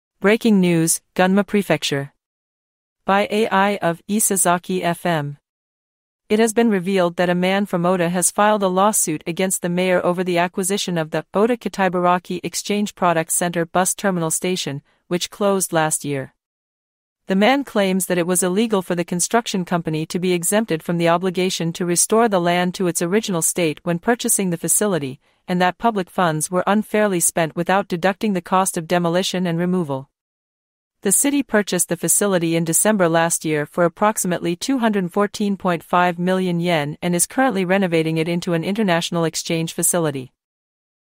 Resident Lawsuit Over Ota Product Exchange Facility Acquisition
Breaking News "Gunma Prefecture".

By AI of "Isesaki FM".

It has been revealed that a man from Ota has filed a lawsuit against the mayor over the acquisition of the "Ota Kitaibaraki Exchange Products Center Bus Terminal Station", which closed last year. The man claims that it was illegal for the construction company to be exempted from the obligation to restore the land to its original state when purchasing the facility, and that public funds were unfairly spent without deducting the cost of demolition and removal. The city purchased the facility in December last year for approximately 214.5 million yen and is currently renovating it into an international exchange facility.